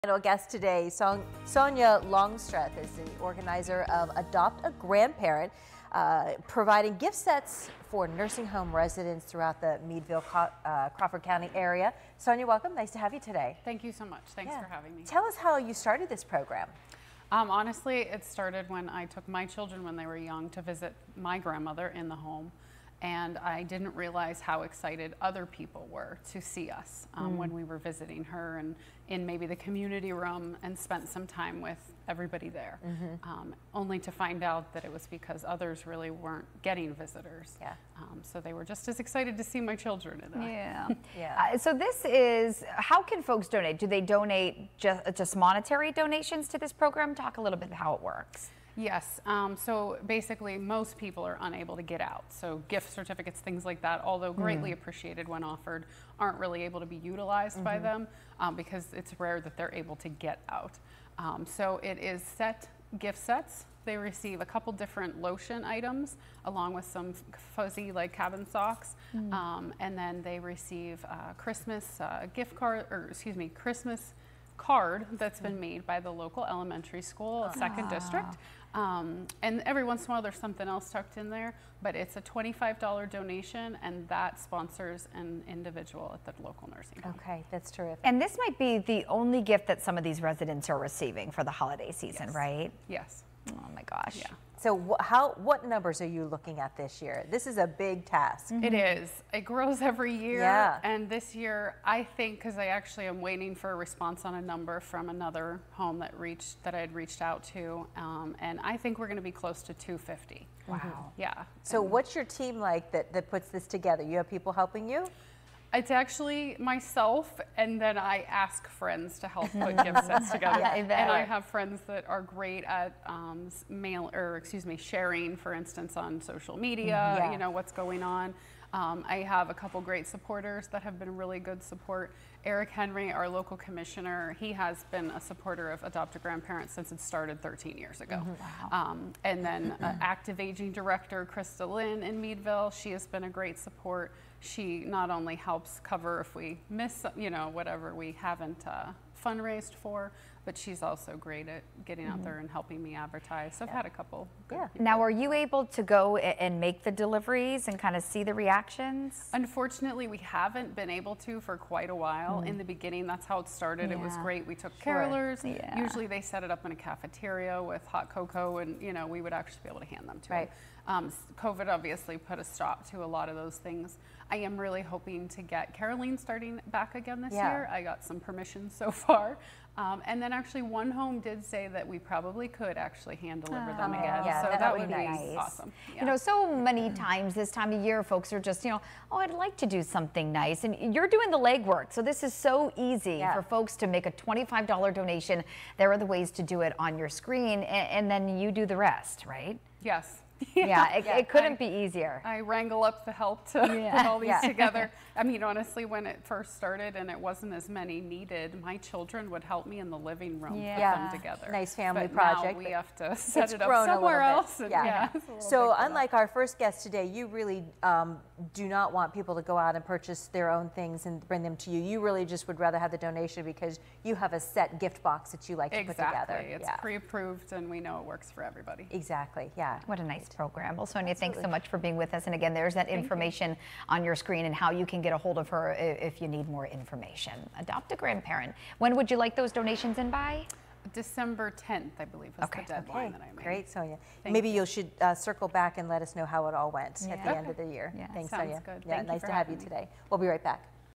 Adopt a Grandparent
Sonya Longstreth discusses Adopt a Grandparent.